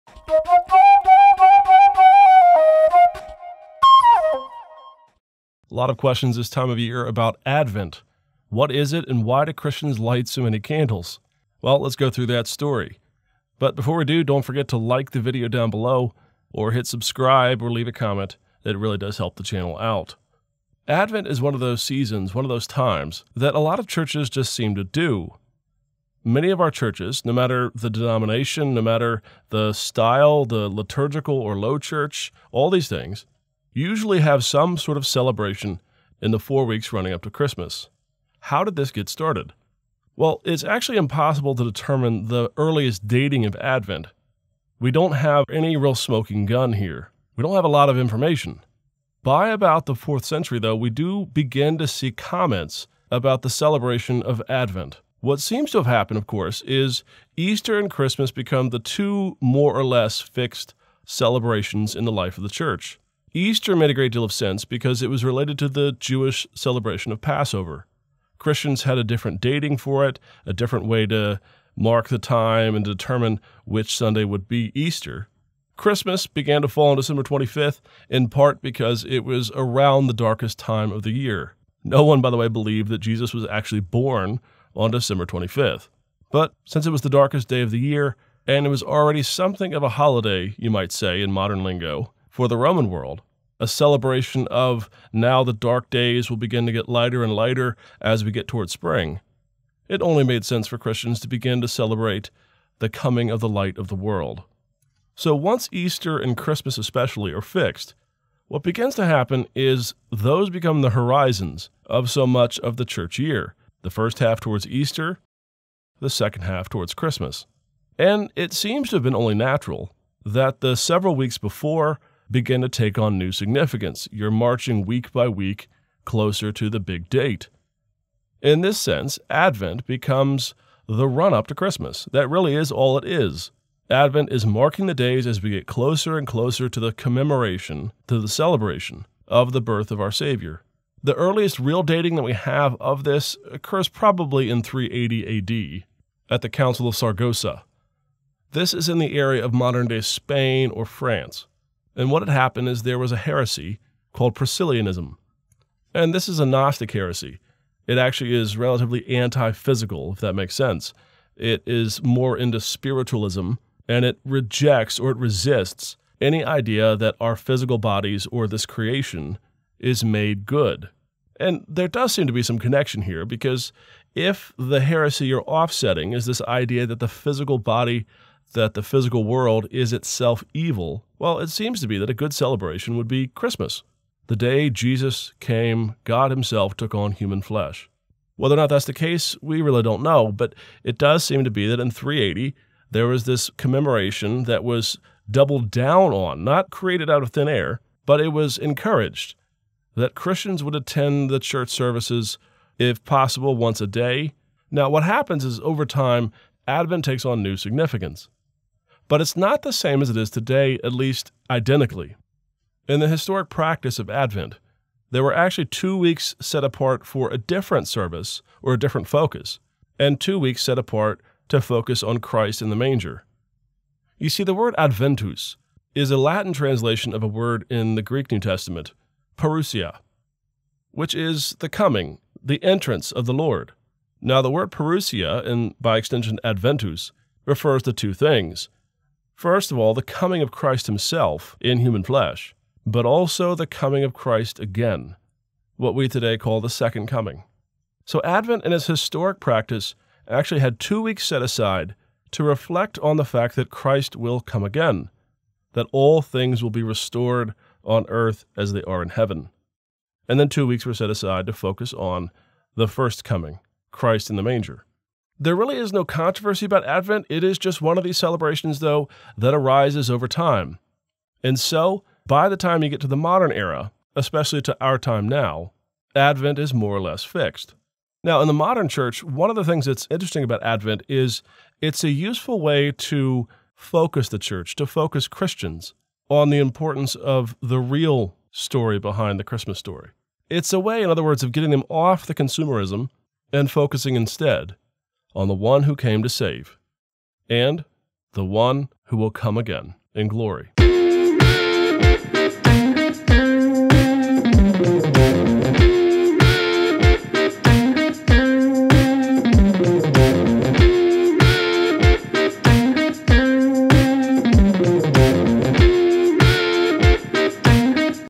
When Did Christians Begin Advent Celebrations?
When in history did the celebration of Advent begin in Christian churches? This short explanation tells how Advent arose around Christmas in the 4th century, and how Advent in history is different than Advent today.

Ryan M. Reeves (PhD Cambridge) is Assistant Professor of Historical Theology at Gordon-Conwell Theological Seminary.  Twitter Instagram